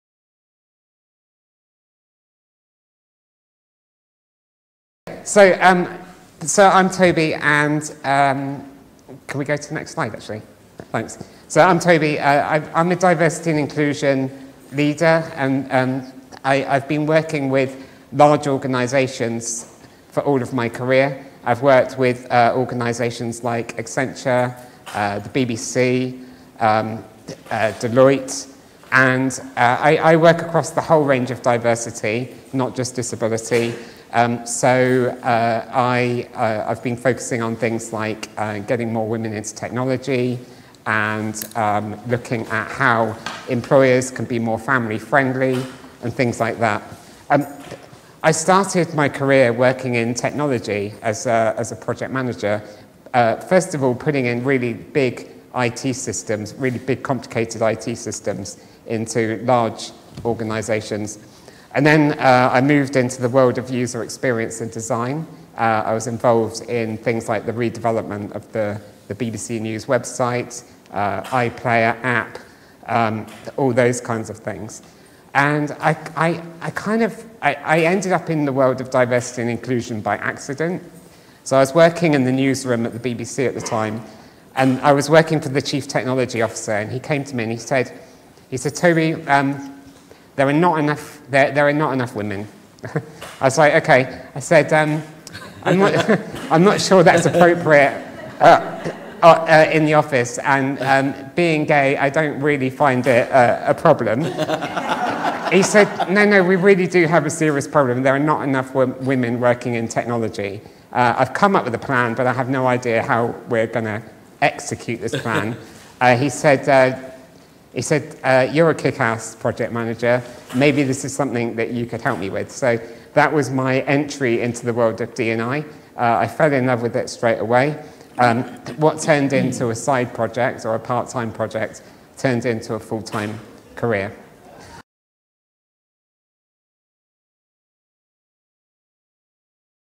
Toby Mildon: Hardwiring Workplace Adjustments
Toby Mildon appearing at Iansyst conference introduces himself and explains a little about his work in diversity and inclusion.